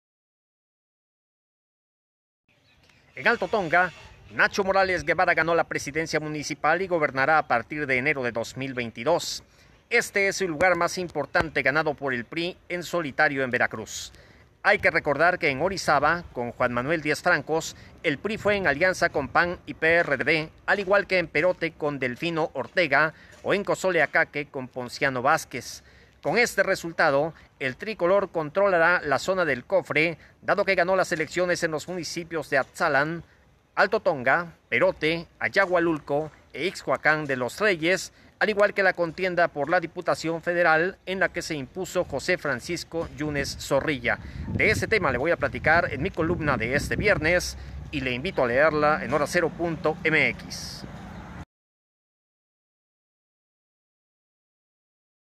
Altotonga, un proyecto regional
Luis Alberto Romero
Con bombo y platillo, el Ayuntamiento de Altotonga presentó, en abril de 2018, el plan municipal de desarrollo, un bodrio de 139 páginas que definía lo que a consideración del gobierno encabezado por Ernesto Ruiz Flández debían ser las estrategias y acciones municipales durante el periodo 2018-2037.